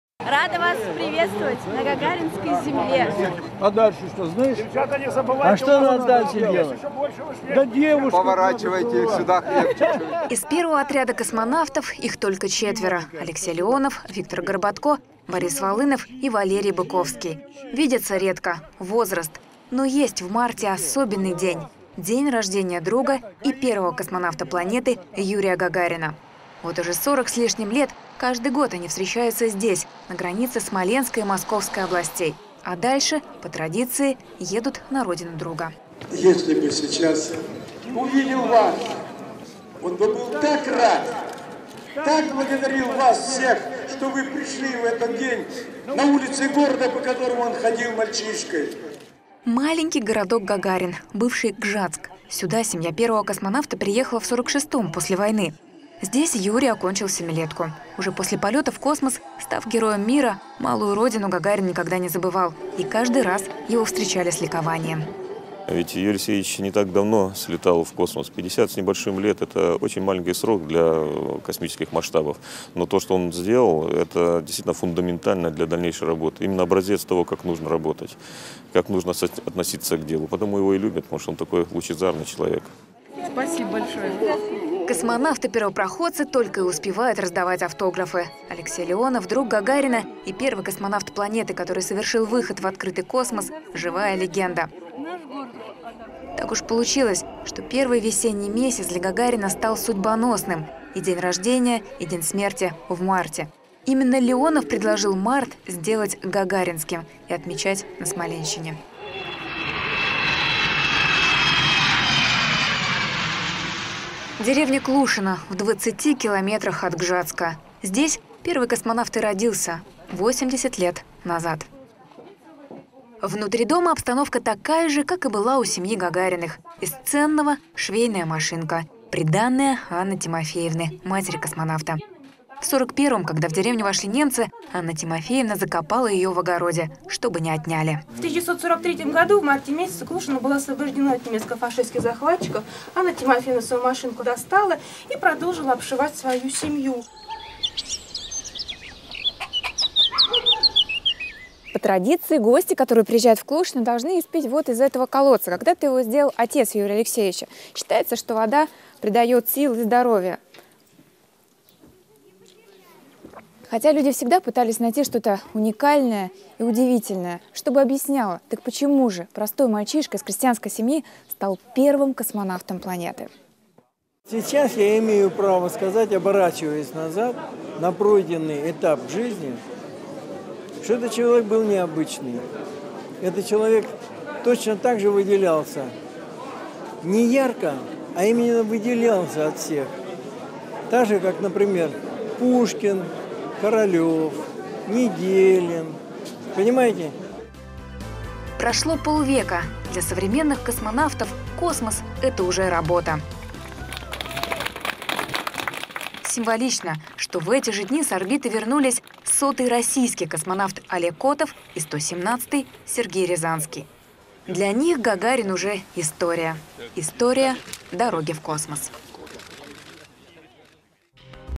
Гагарин: полёт в будущее
Вторая неделя марта началась под знаком космоса. В стране отметили 80-летие космонавта №1 - Юрия Гагарина. На Смоленской земле, родине первопроходца космоса прошли торжественные мероприятия, в которых приняли участие космонавты разных поколений. Символично, что в эти же дни на Землю вернулся экипаж «Союза», проработавший на орбите полгода.